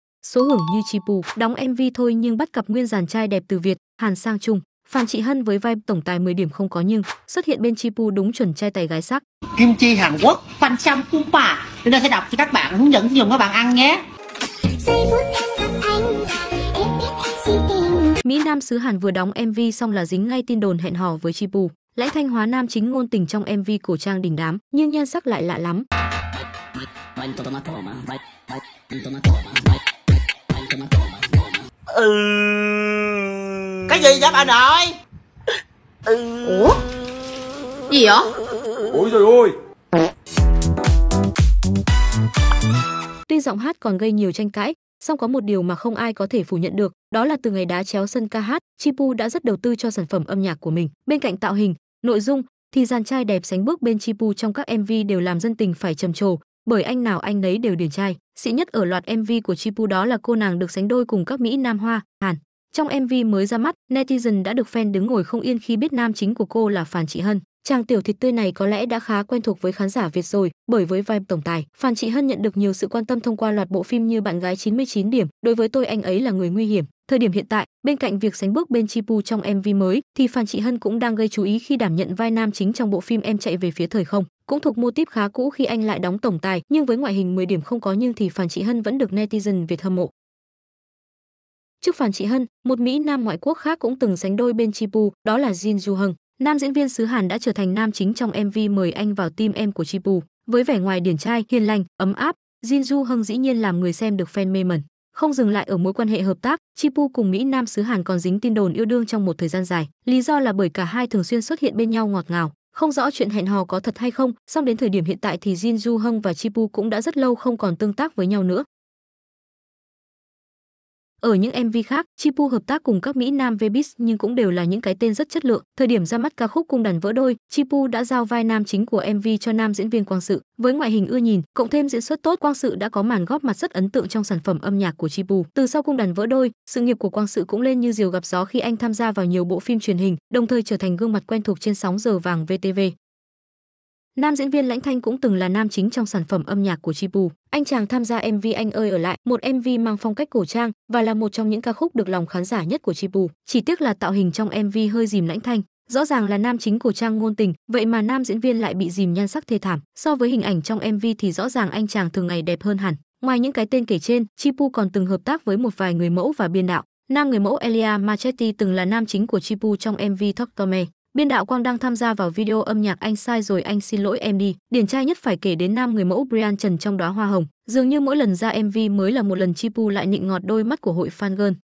Số hưởng như Chi Pu: Đóng MV thôi nhưng bắt cặp nguyên dàn trai đẹp từ Việt, Hàn sang Trung
Tuy giọng hát còn gây nhiều tranh cãi, song có một điều mà không ai có thể phủ nhận được, đó là từ ngày đá chéo sân ca hát, Chi Pu đã rất đầu tư cho sản phẩm âm nhạc của mình. Bên cạnh tạo hình, nội dung, thì dàn trai đẹp sánh bước bên Chi Pu trong các MV đều làm dân tình phải trầm trồ, bởi anh nào anh nấy đều điển trai.